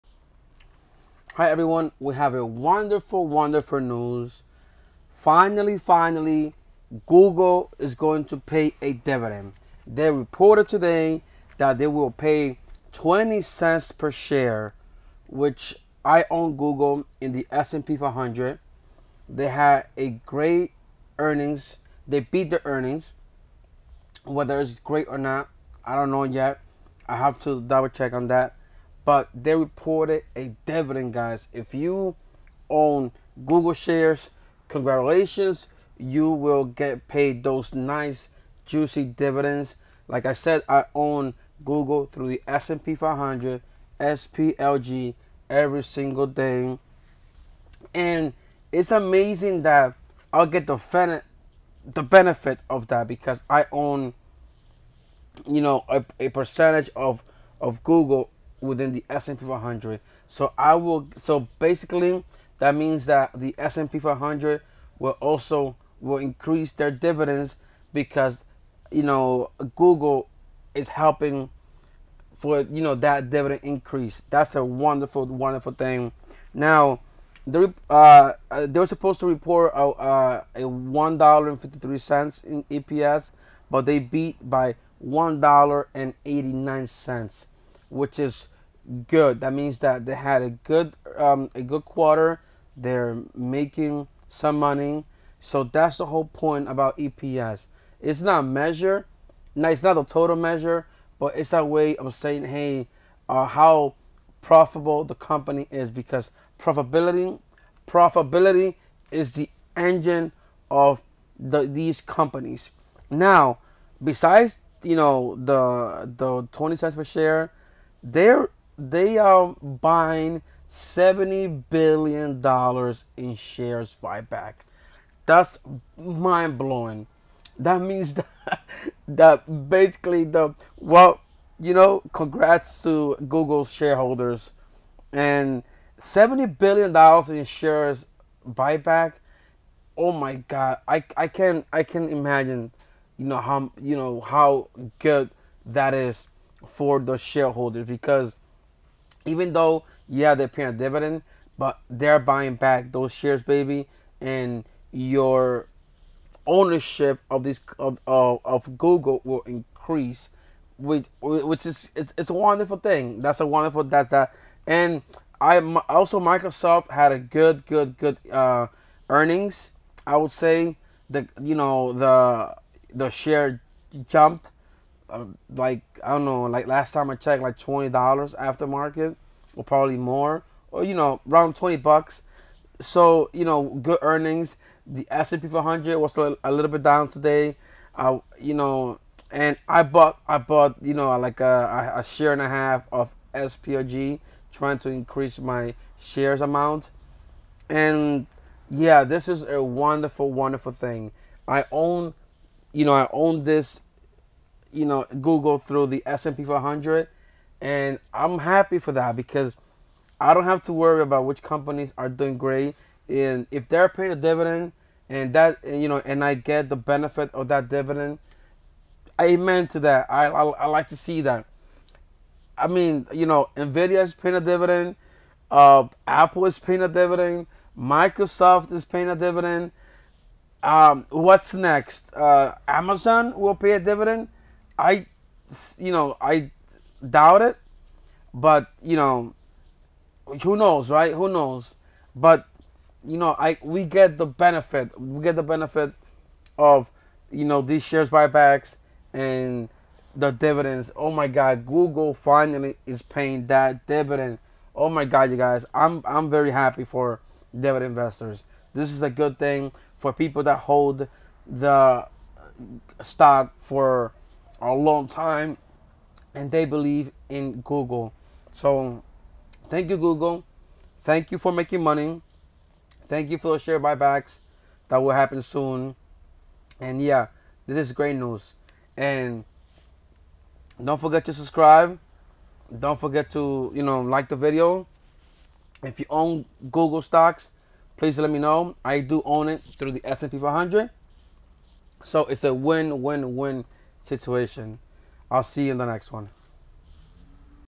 Google Stock Will Pay A Dividend
Alphabet (GOOGL, GOOG) declares a cash dividend of $0.20 per share to investors while reporting a first-quarter earnings beat; the Google parent company topped expectations on the top and bottom lines, posting adjusted earnings of $1.89 per share (against estimates of $1.53) and $67.59 billion (against estimates of $66.07 billion). And Alphabet's stock is overjoyed, surging as high as 13% in Thursday's after-hours trading session.